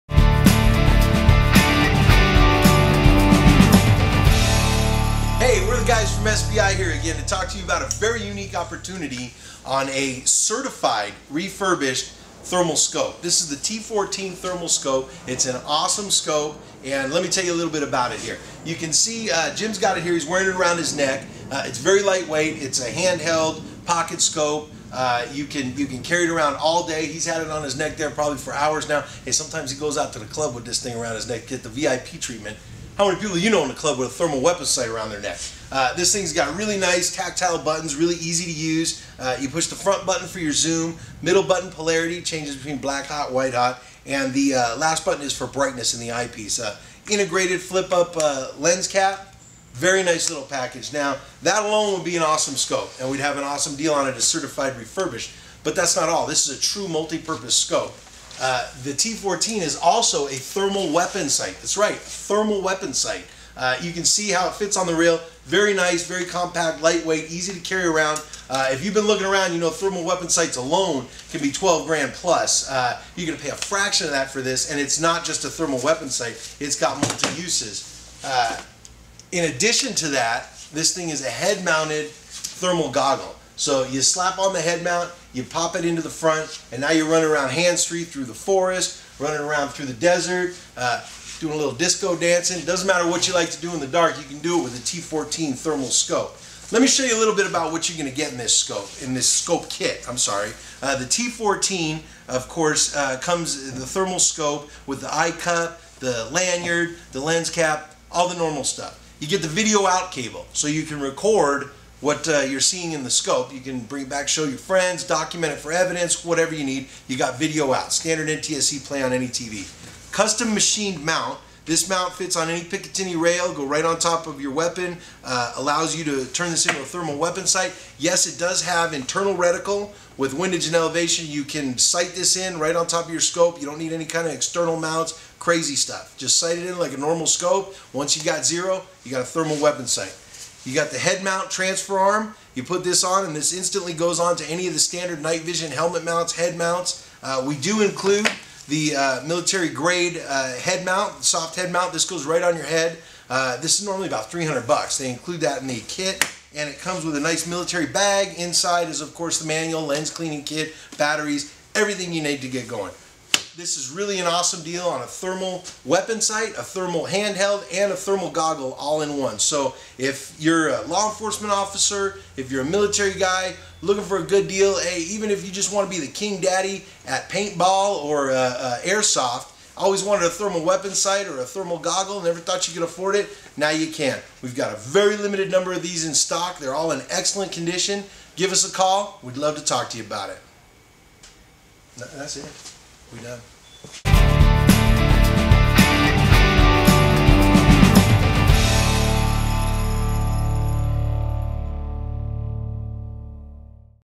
T14 Thermal Weapon Sight - Thermal Scope
The T14 is a very unique multipurpose thermal scope. The T14 can be used as a handheld thermal imager AKA Pocket Scope. The T14 is also a very capable thermal weapon sight that can be mounted to a rifle as a thermal scope. Finally the T14 can be mounted to standard night vision head mounts and helmet mounts to be used as a hands free thermal goggle device. SPI has a limited supply of certified refurbished T14 thermal imagers in stock at a small fraction of regular price.  This is a once in a lifetime chance to acquire a low cost thermal imager or thermal rifle scope. Even if you just always wanted a thermal goggle or thermal rifle scope for playing paintball or airsoft and thought that you could never afford one.  Now you can own an infrared imaging system for much less than you think.